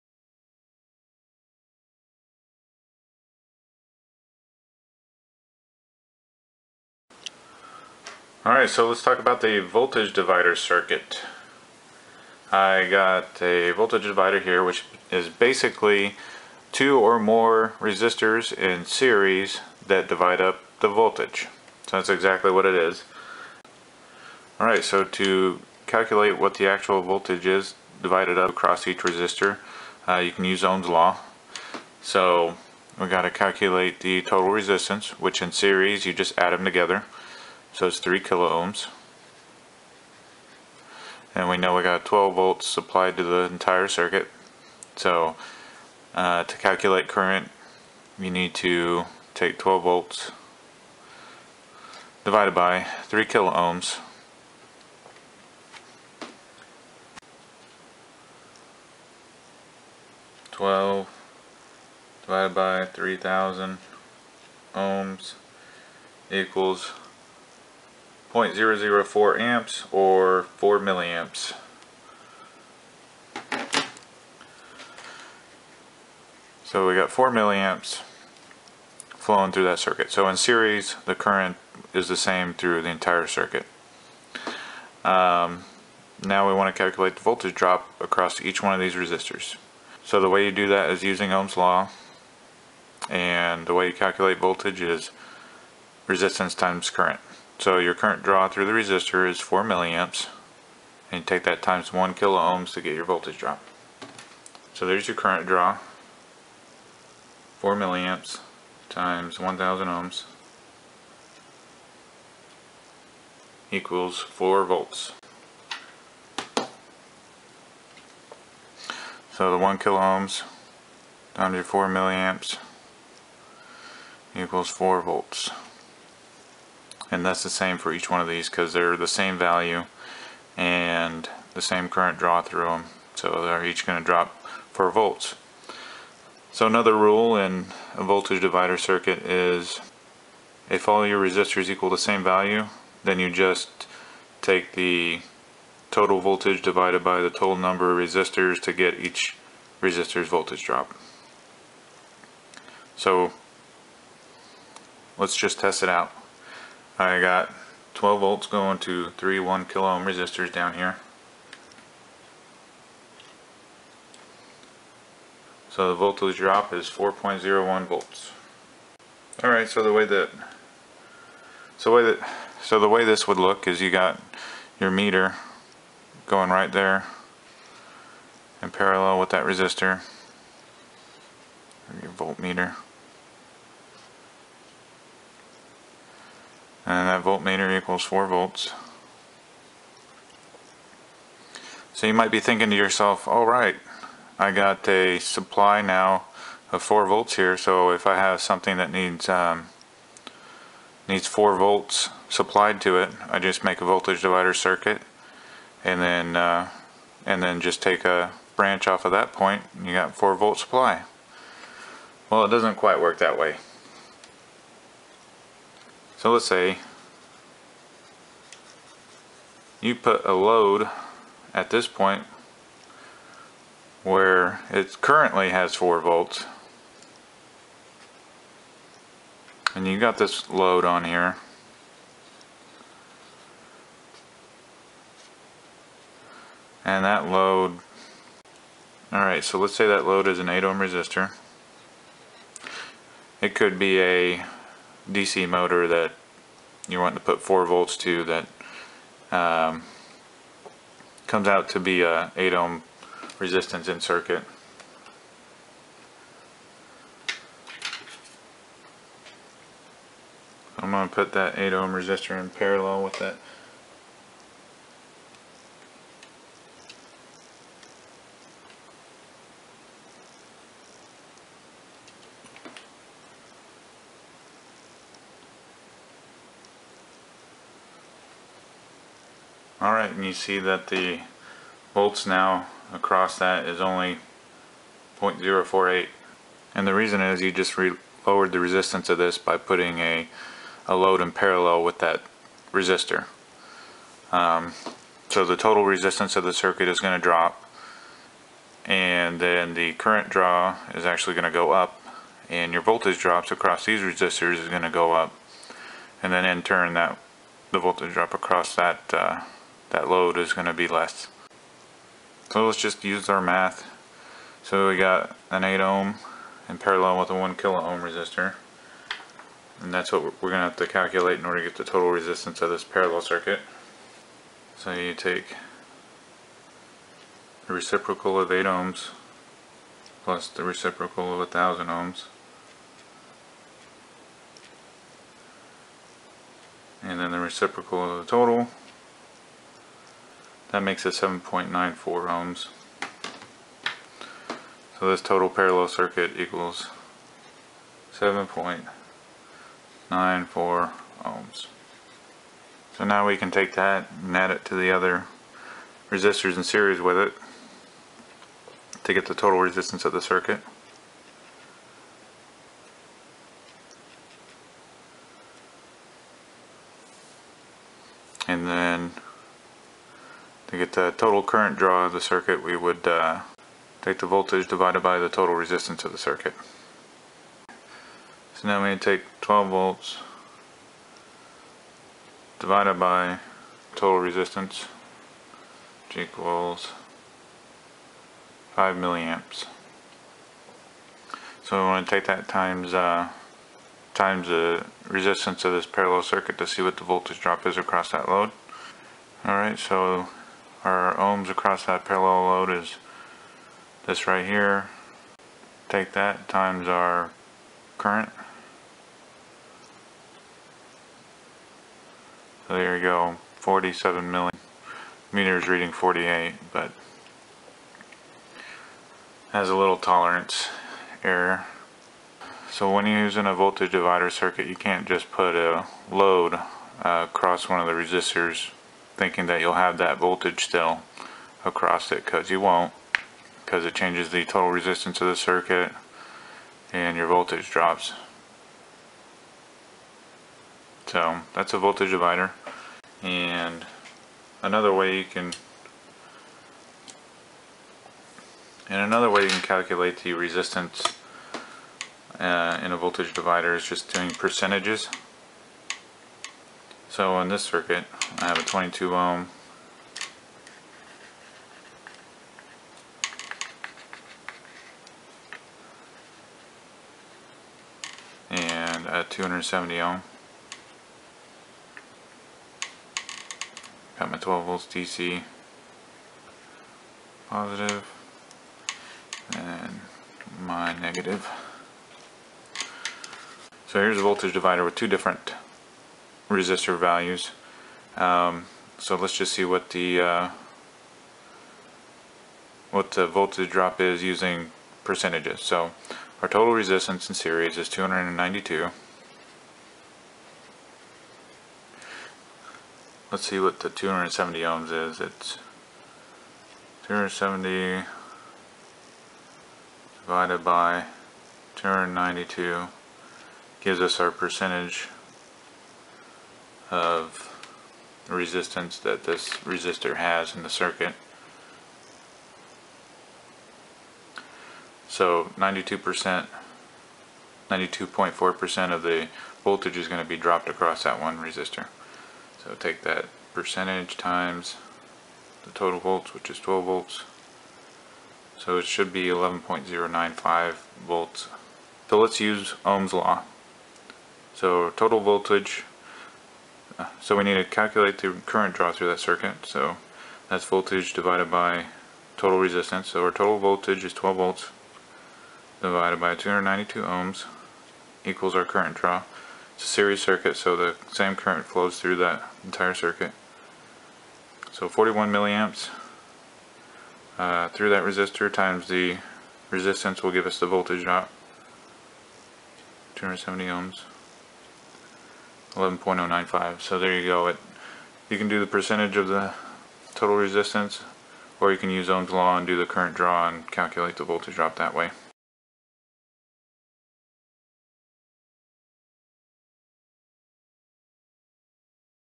Voltage Divider Circuit
Show what a voltage divider is and how to calculate the divided up voltage.